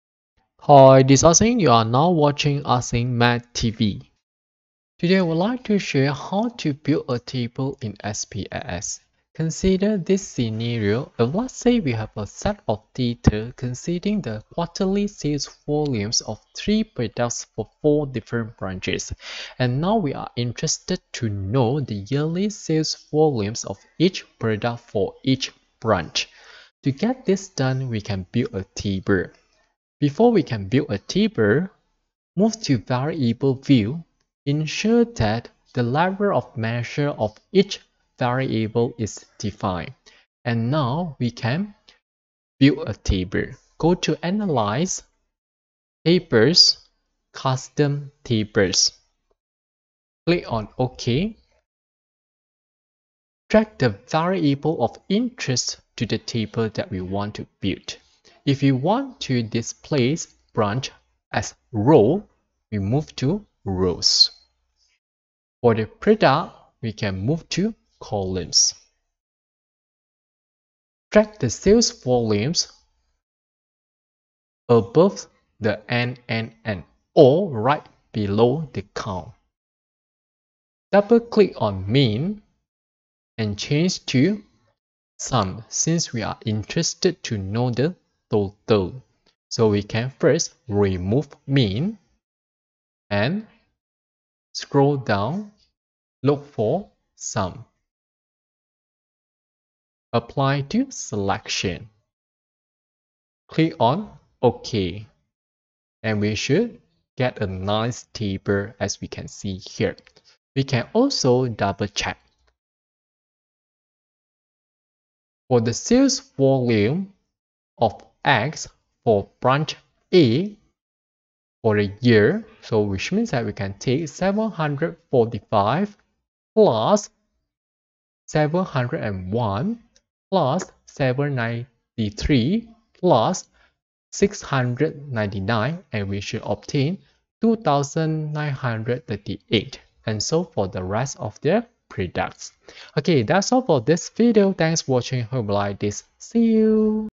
How to create table in SPSS
IBM SPSS Lesson 3: Creating or constructing tables

Steps:
1. Analyze
2. Tables
3. Custom Tables
4. Design table
5. Select the statistics to be presented